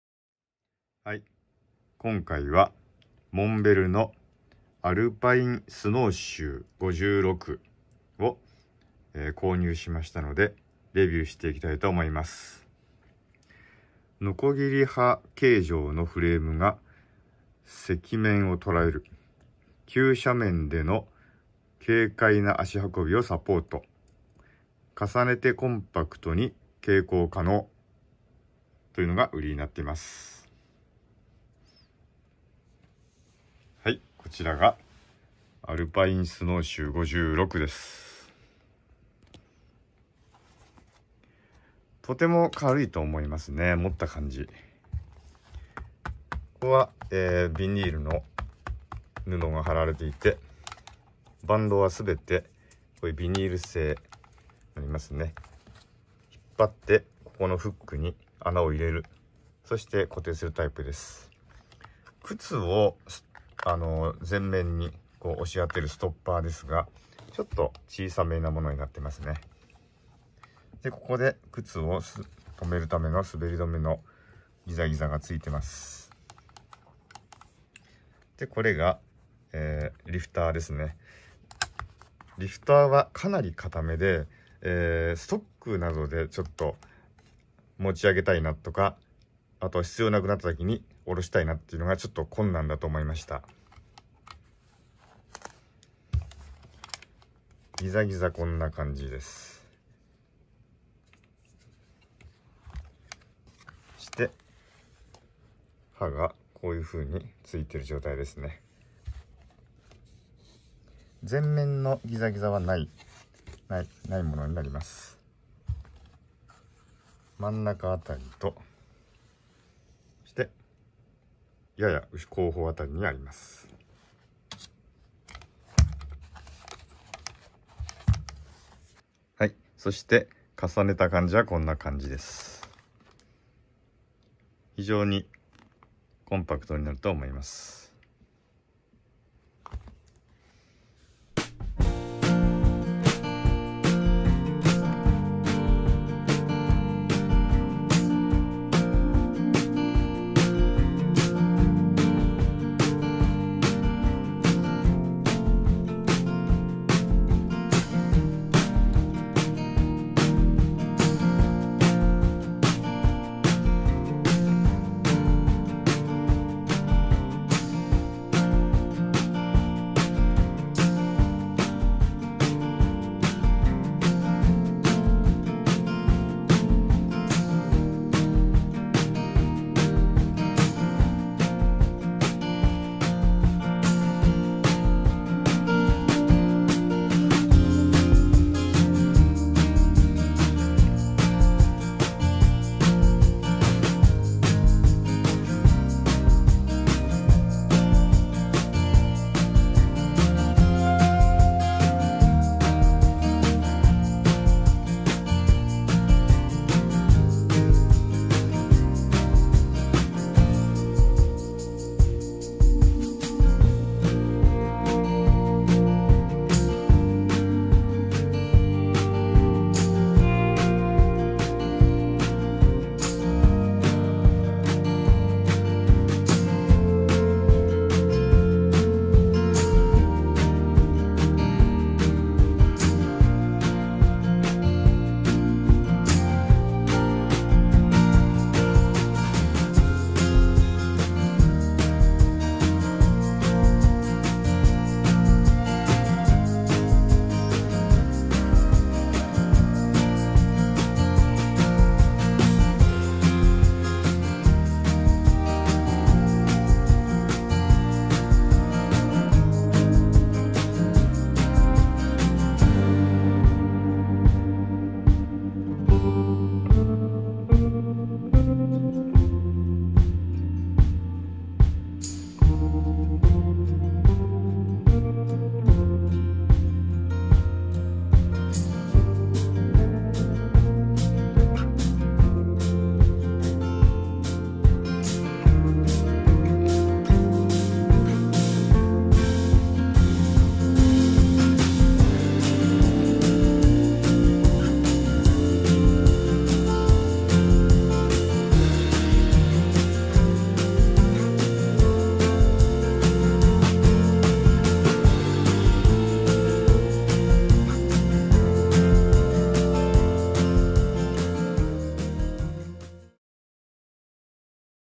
モンベル アルパインスノーシュー56
モンベルのスノーシューを買ったので紹介します